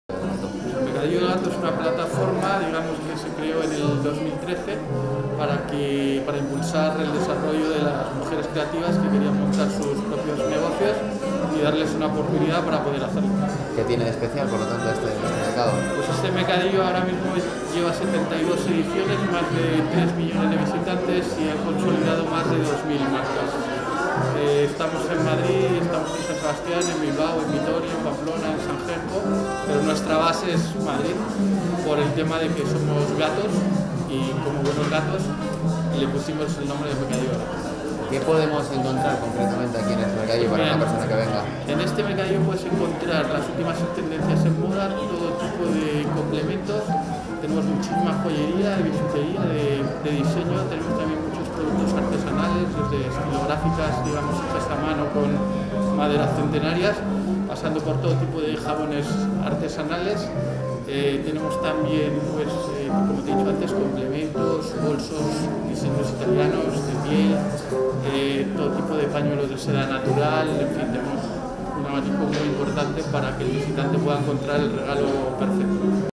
El Mercadillo del Gato abre sus puertas con productos selectos
El más famoso mercadillo pop up de la capital regresa para ofrecer piezas únicas al público. Más de 60 expositores con productos selectos de moda, artesanía, gourmet, decoración, complementos, moda infantil, cosmética, bisutería y coleccionismo.